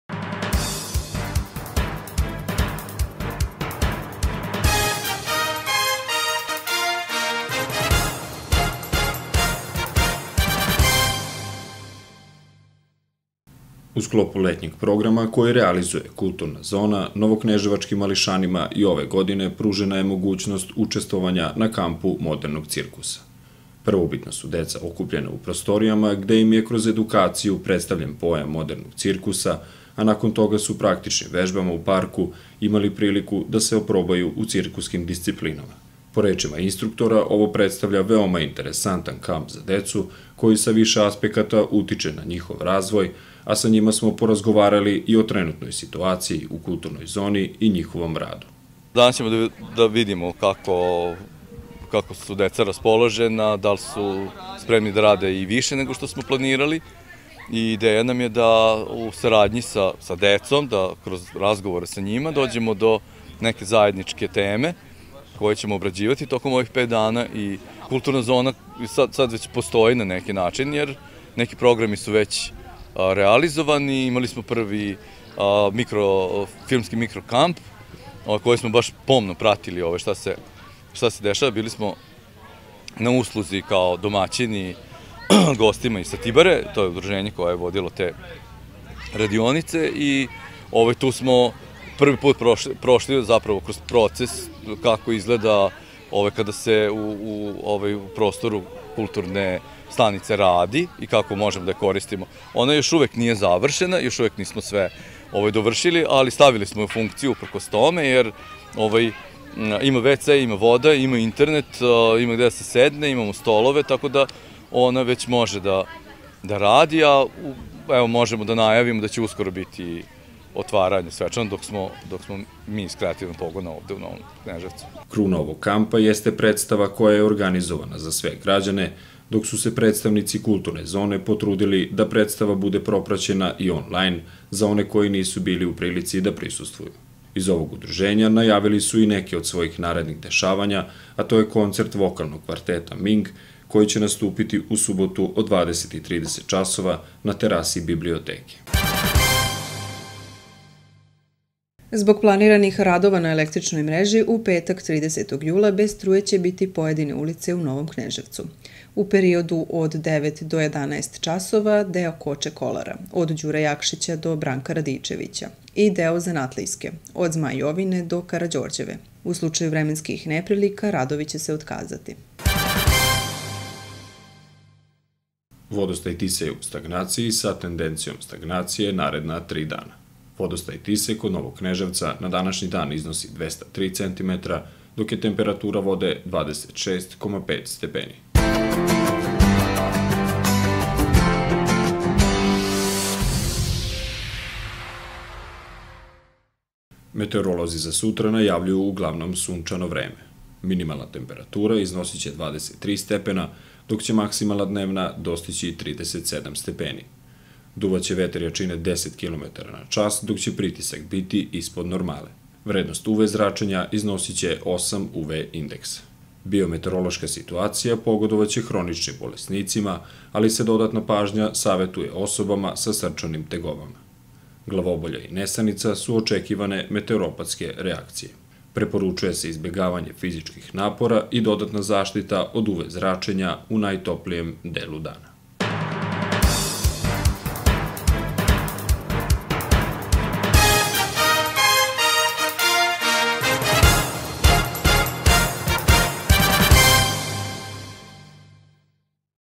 NK Vesti 29. 7. 2021.
U sklopu letnjeg programa koji realizuje Kulturna Zona novokneževačkim mališanima i ove godine pružena je mogućnost učestvovanja na kampu modernog cirkusa. Prvobitno su deca okupljena u prostorijama gde im je kroz edukaciju predstavljen pojam modernog ciruksa a nakon togu su praktičnim vežbama u parku imali priliku da se oprobaju u cirkuskim disciplinama. Po rečima instruktora ovo predstavlja veoma interesantan kamp za decu koji sa više aspekata utiče na njihov razvoj a sa njima smo porazgovarali i o trenutnoj situaciji u Kultirnoj Zoni i njohovom radu

  Kruna ovog kampa jeste predstava koja je organizovana za sve građane, dok su se predstavnici Kulturne Zone potrudili da predstava bude propraćena i onlajn za one koji nisu u prilici da prisustvuju. Iz ovog udruženja najavili su i neke od svojih narednih dešavanja a to je koncert vokalnog kvarteta Ming koji će nastupiti u subotu od 20:30h na terasi biblioteke.